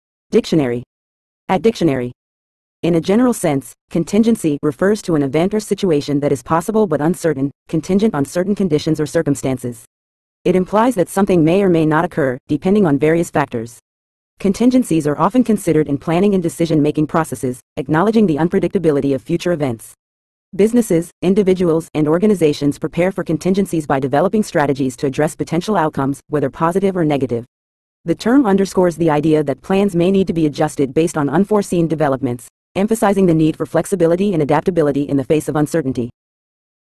Contingency Meaning In English
In a general sense, "contingency" refers to an event or situation that is possible but uncertain, contingent on certain conditions or circumstances. It implies that something may or may not occur, depending on various factors. Contingencies are often considered in planning and decision-making processes, acknowledging the unpredictability of future events. Businesses, individuals, and organizations prepare for contingencies by developing strategies to address potential outcomes, whether positive or negative. The term underscores the idea that plans may need to be adjusted based on unforeseen developments, emphasizing the need for flexibility and adaptability in the face of uncertainty.